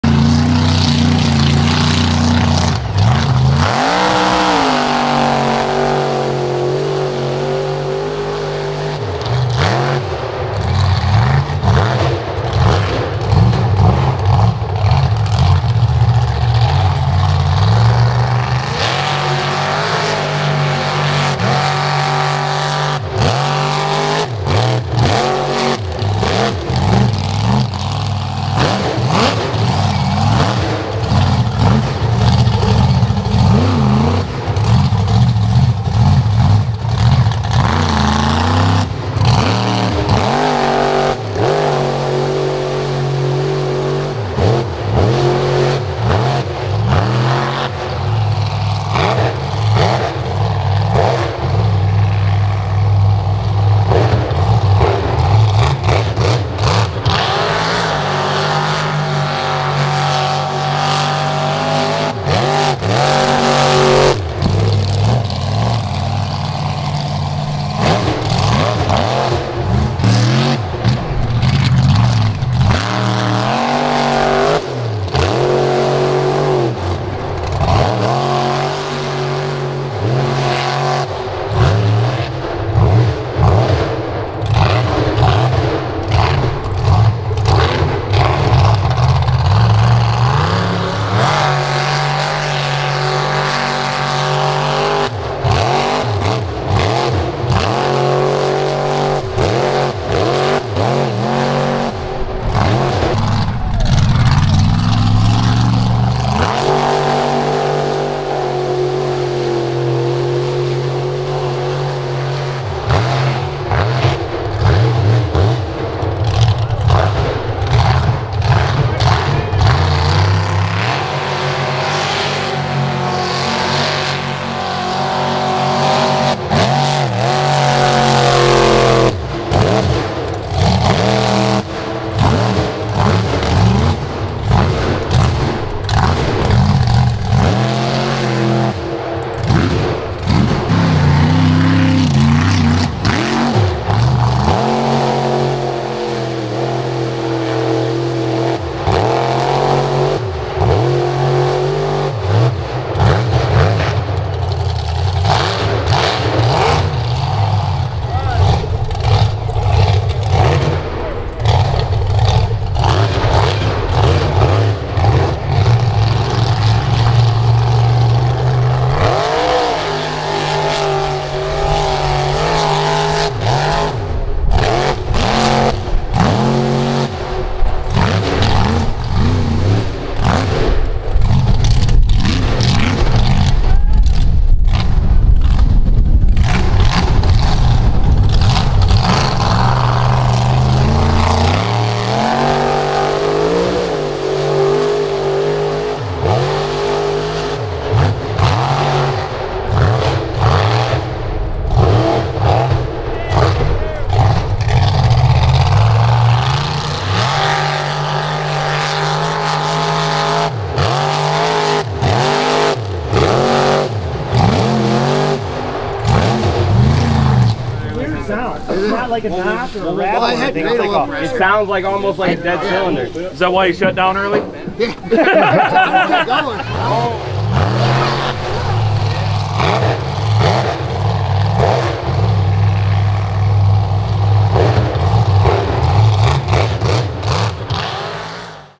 Full Blown Mega Truck
Watch Full Blown Mega Truck take on the black peat mud. Just when you think he's done...nope back for more! Cooley was on a mission that day.